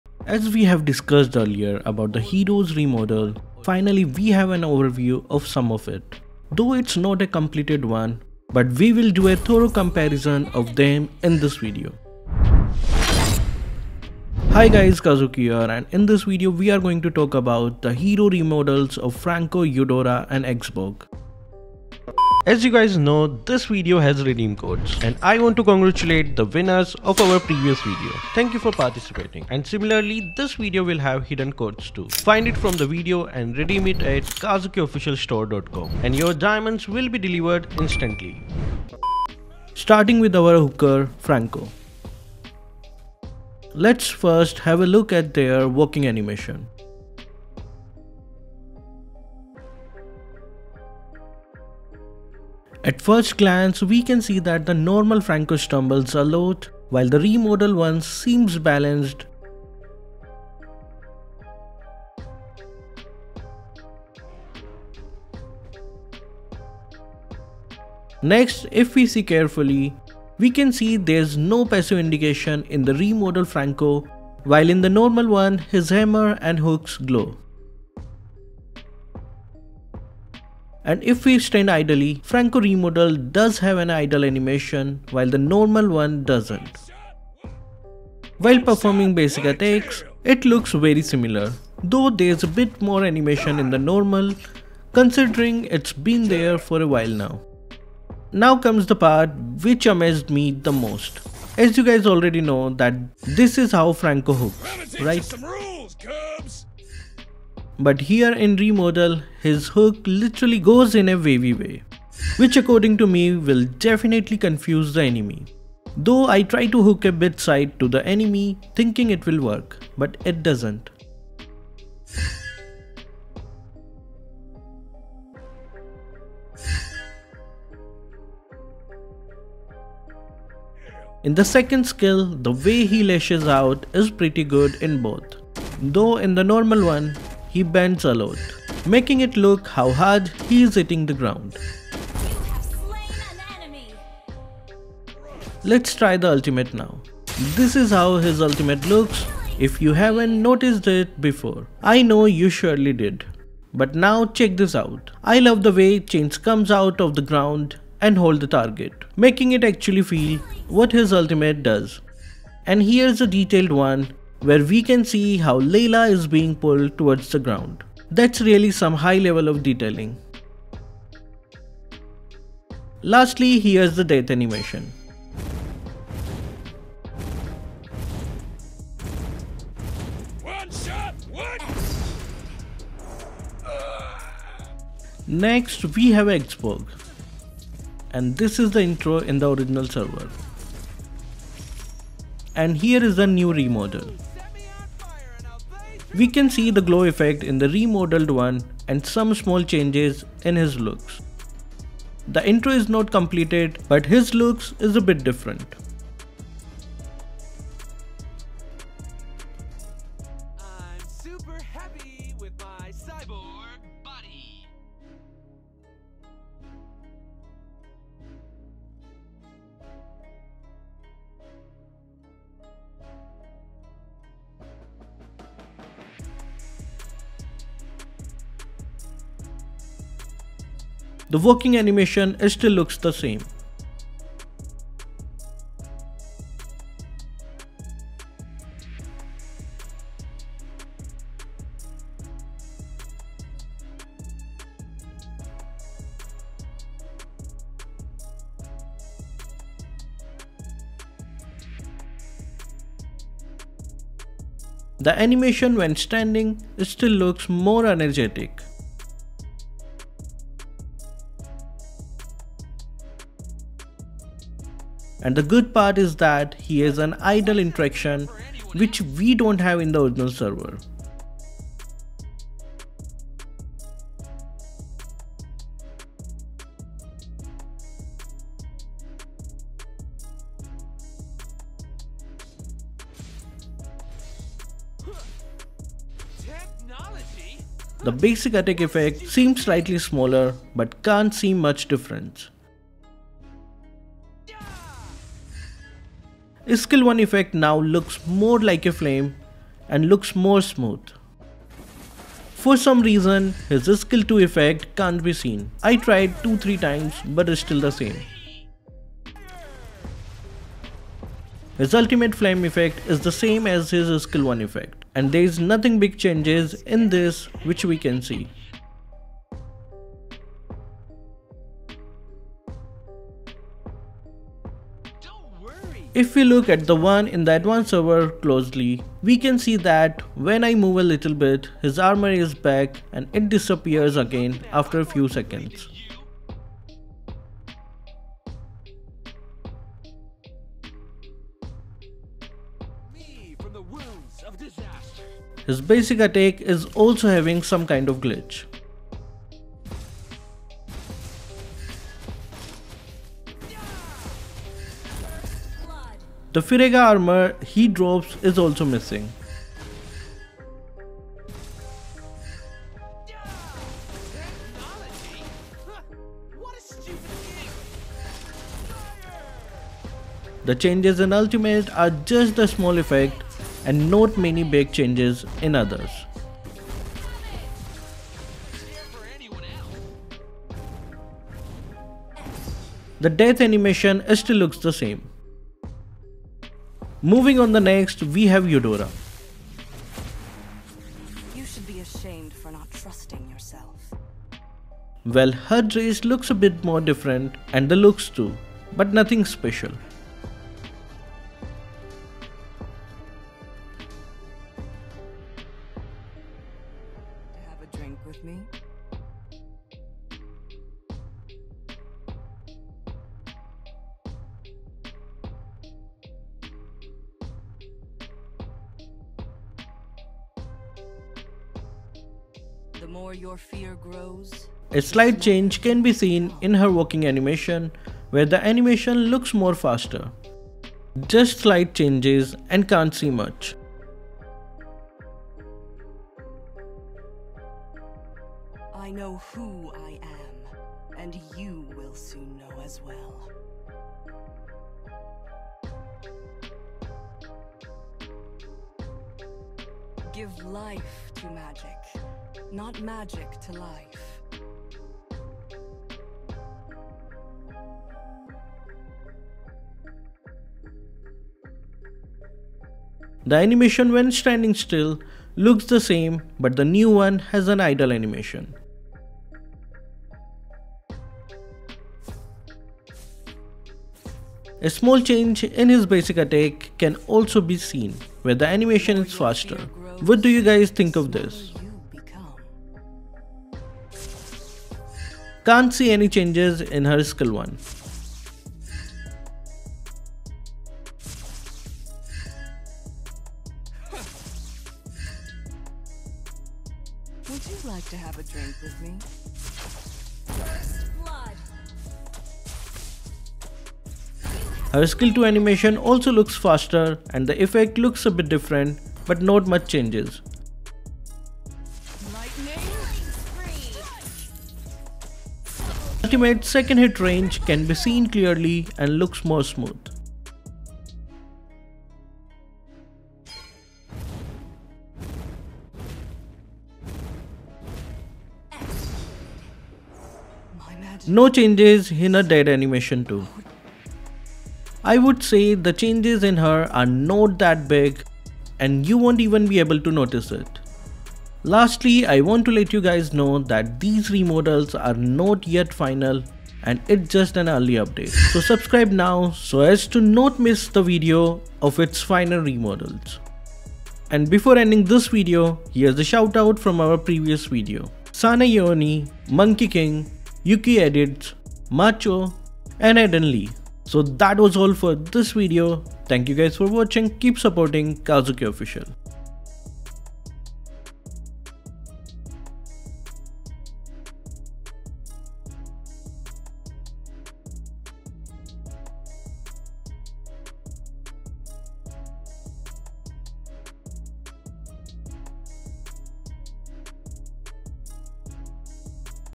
UPCOMING MAJOR HERO REVAMP | FRANCO REVAMP | EUDORA REVAMP & X BORG REVAMP
This video is about the Hero Remodels of Franco, Eudora & X-Borg.